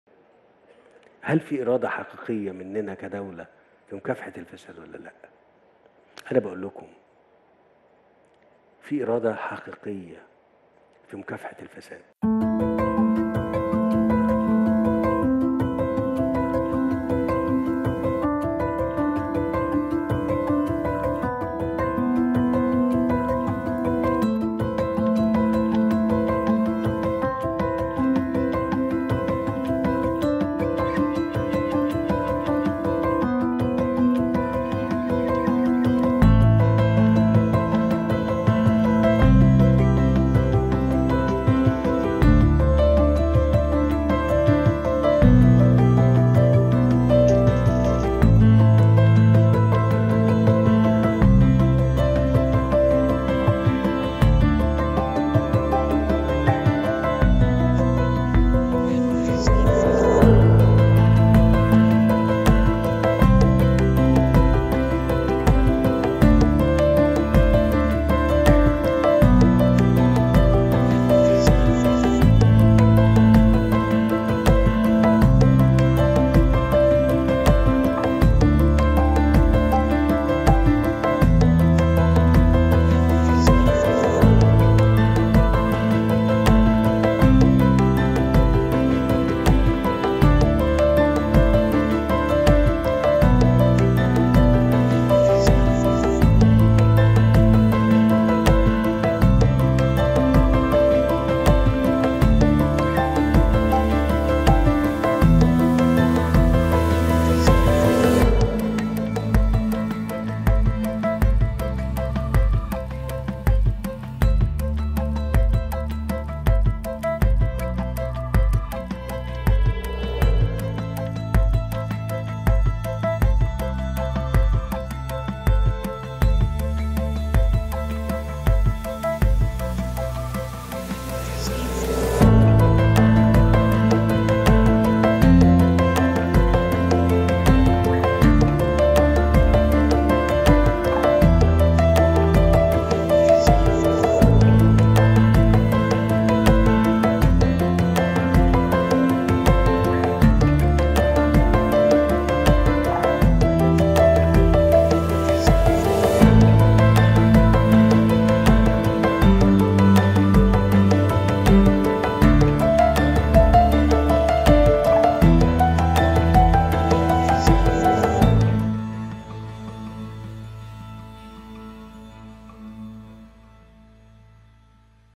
الحرب على الفساد.. كيف سقط المسؤولون
"فيه إرادة حقيقية لمكافحة الفساد"
قضايا كبرى تضمنت أسماء وزراء ومسؤولين منذ 2015 وحتى الآن
هل تحقق استراتيجية مكافحة الفساد أهدافها؟